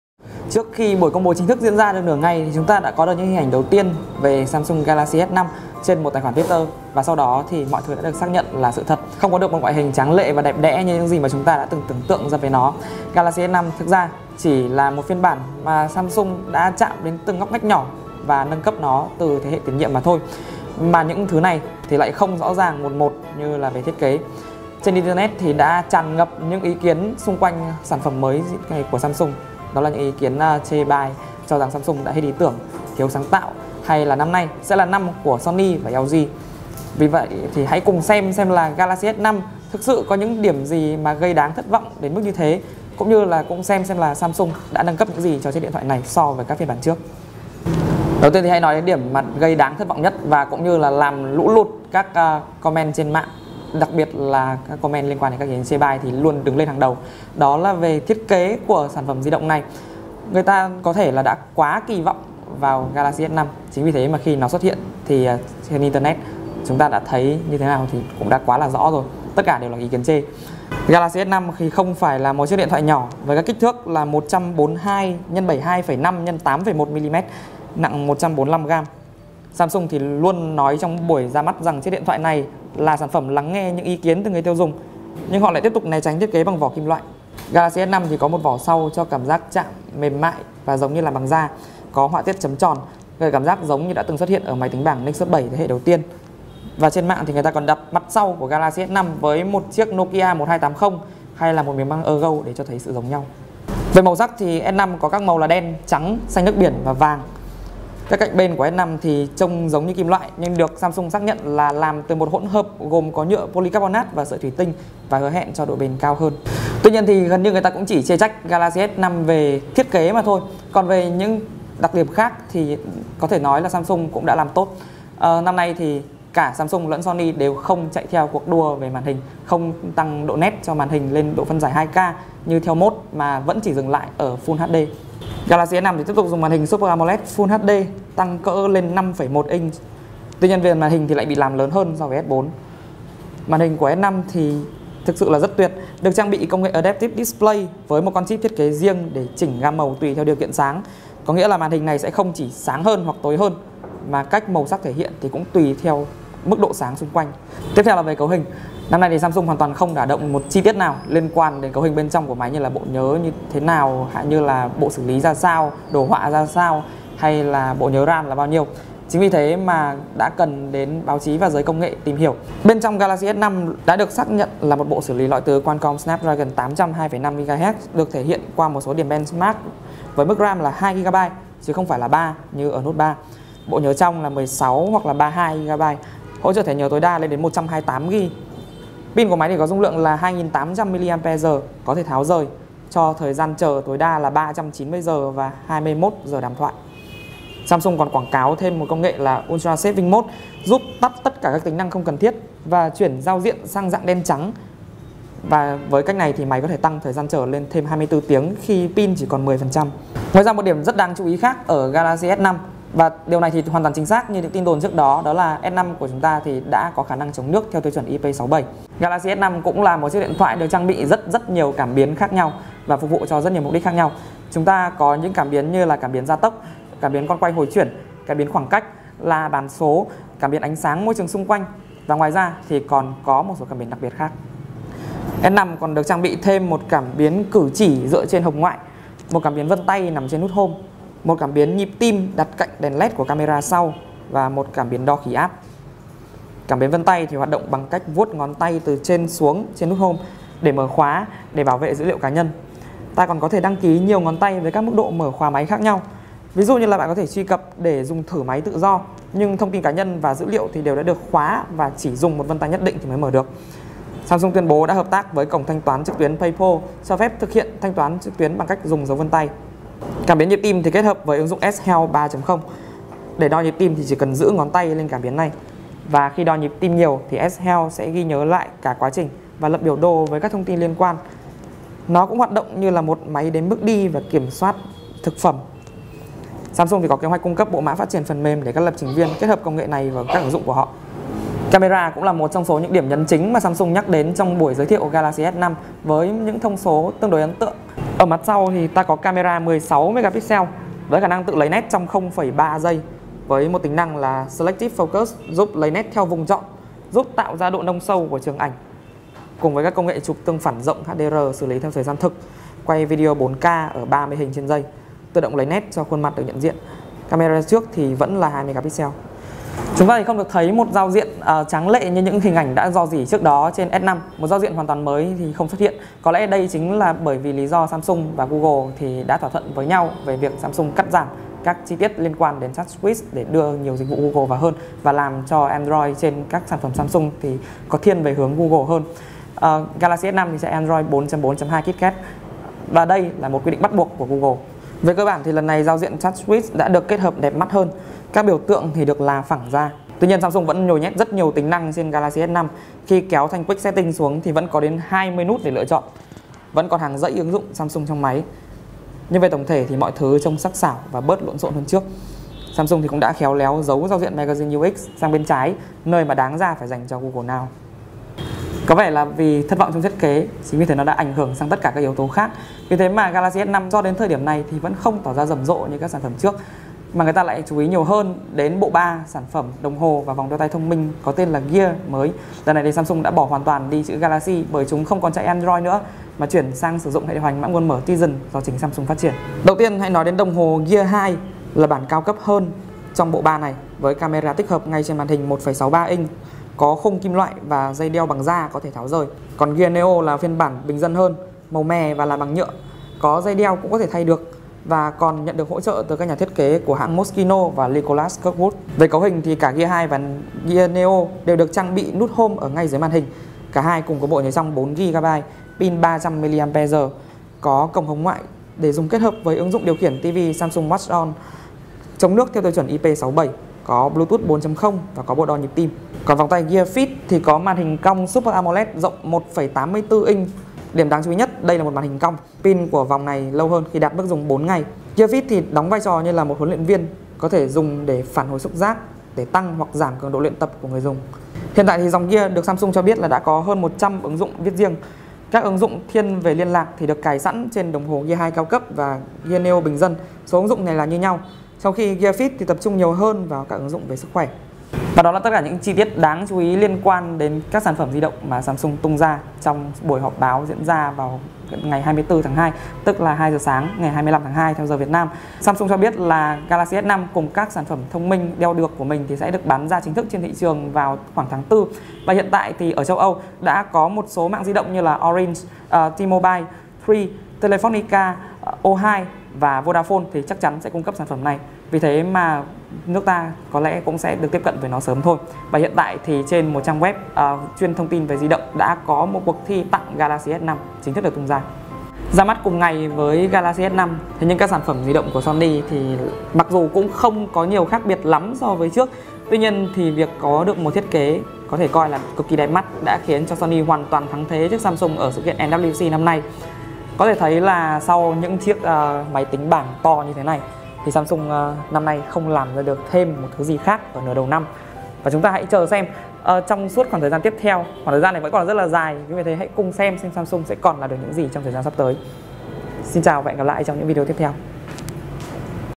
Một số tính năng vượt trội của Samsung Galaxy S5 & Gear
Clip: Giới thiệu tính năng của Samsung Galaxy S5 & Gear
Ngôn ngữ: Việt Nam

Một số tính năng

1. Gear Fit
2. Camera
3. Thời lượng Pin
4. Khả năng chống nước
5. Cảm biến đo nhịp tim
5. ...vv